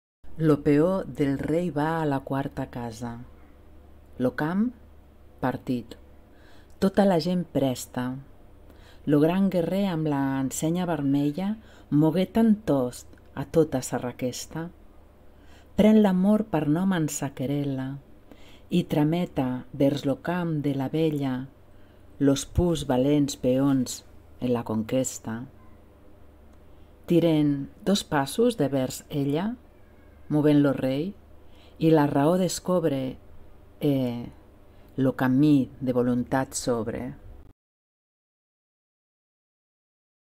Macedonia d'Art en Confitura 2023 08 20 6818 Neus Silvestre
Video de Poesia  de Francesc Castellvi 1475 amb pintura i veu de Neus Silvestre - Col.lectiu d'Artistes  de Sants